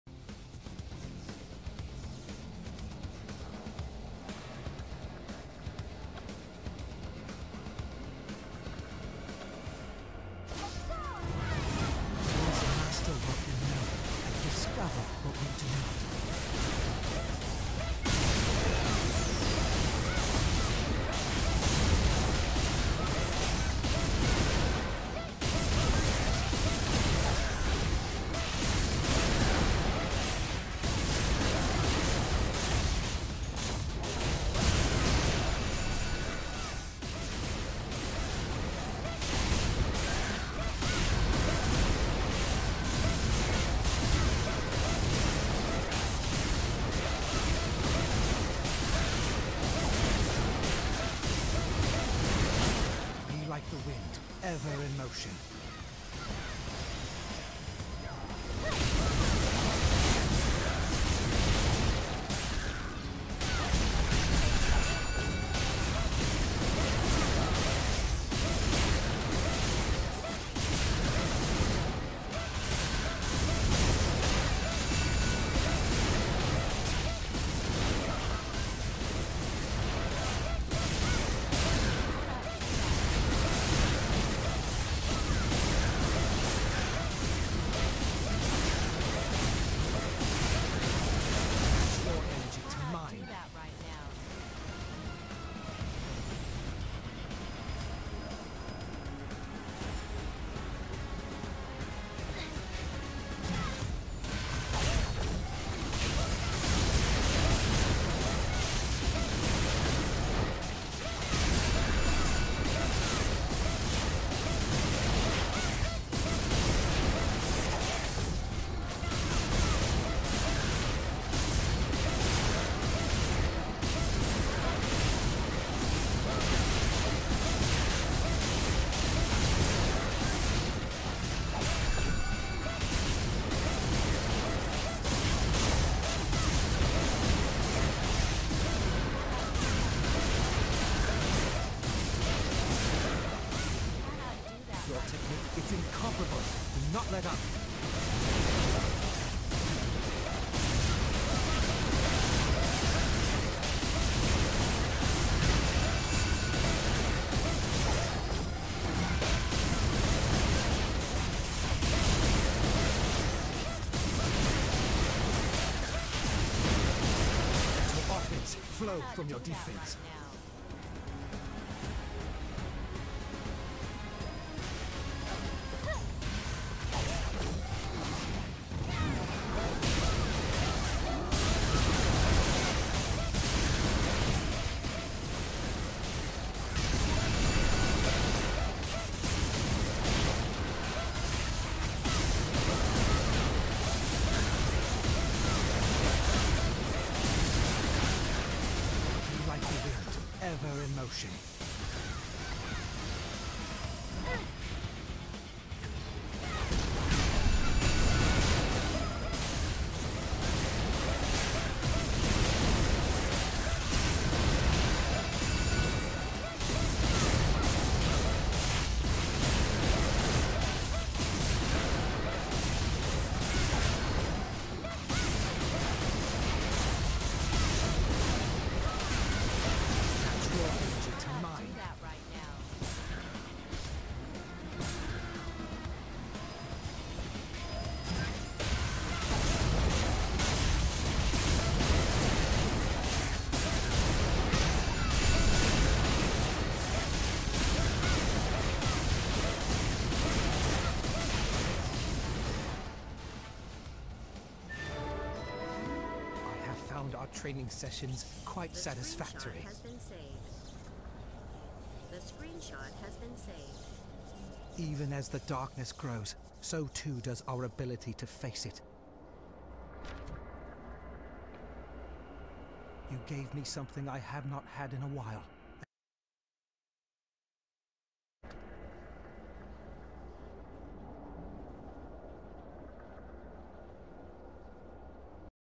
Shrouded Ajanara - Hard, Sanctified
Not even close baby!

I haven't watched any guides :D

This boss is more like a dps check tho. It's challenging and exciting, but I liked Eva better. Still good boss tho :)

reupload